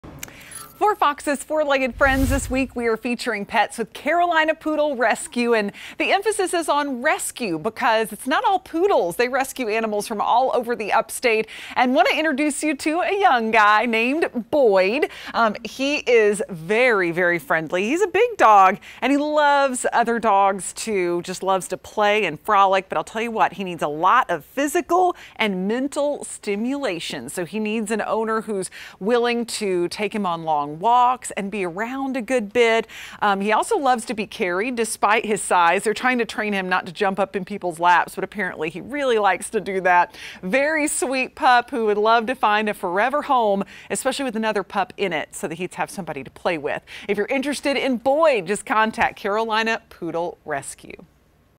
Four Legged Friends: Boyd
Boyd is being held with Carolina Poodle Rescue.